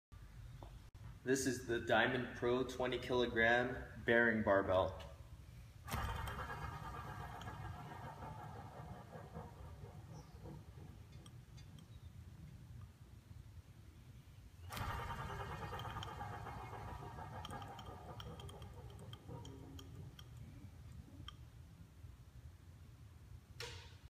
Diamond Pro Select 20kg Olympic Bearing Barbell Spin Test
165k Tensile
8 Bearing (4 per sleeve)
Black Zinc/Bright Zinc
Light knurling